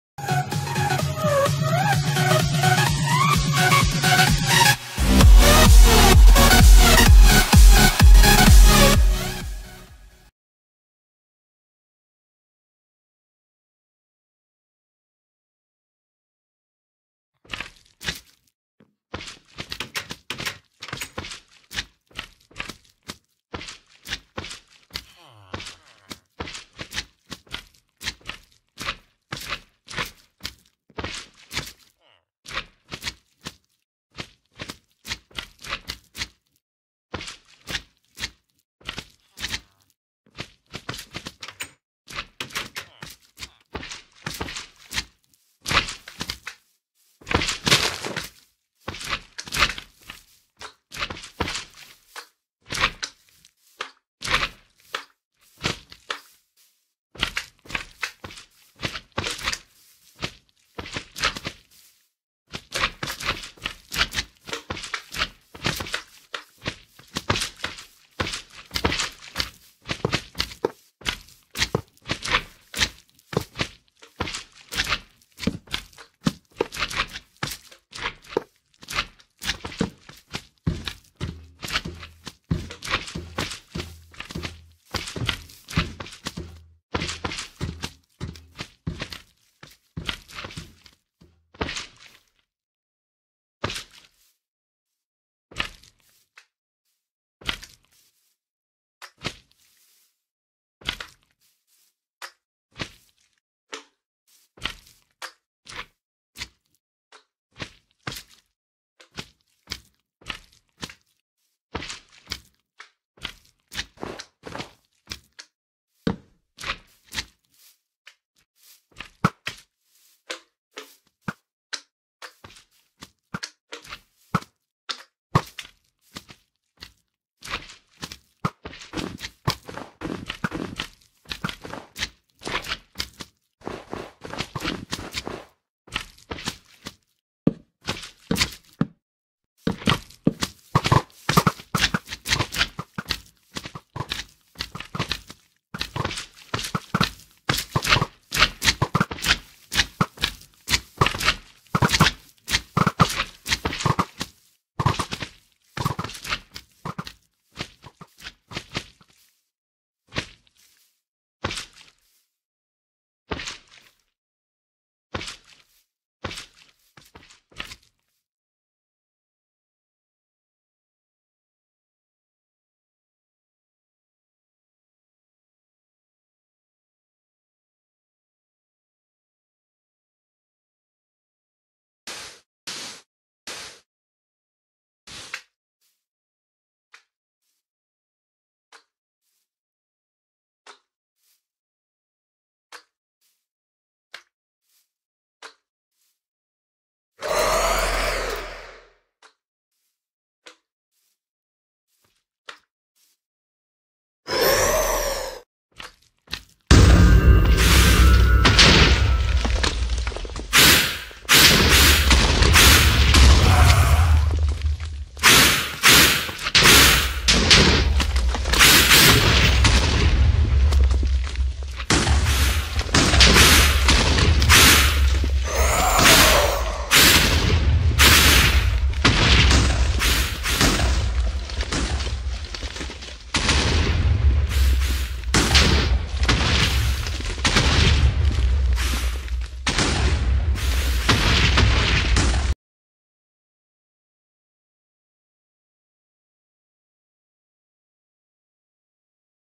How to spawn wither/iron golem/snow golem
Hello
for wither you will need;4 soulsand and 3 wither skeleton skull
     iron golem you will need;5 blocks of iron and 1 jack o lantern
     snowman u will need;2 block of snow and 1 jack o lantern
Have fun yea!!